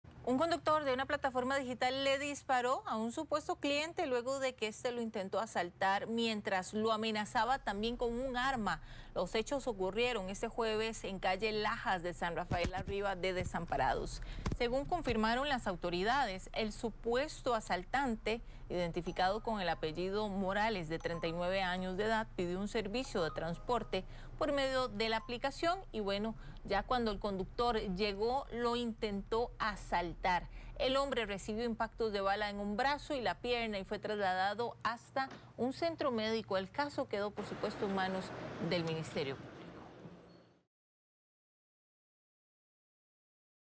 Conductor de plataforma digital se defiende de asaltante en San Rafael Arriba de Desamparados
En San Rafael Arriba de Desamparados, un conductor de plataforma se defendió de un presunto cliente que lo intentó asaltar mientras lo amenazaba con un arma.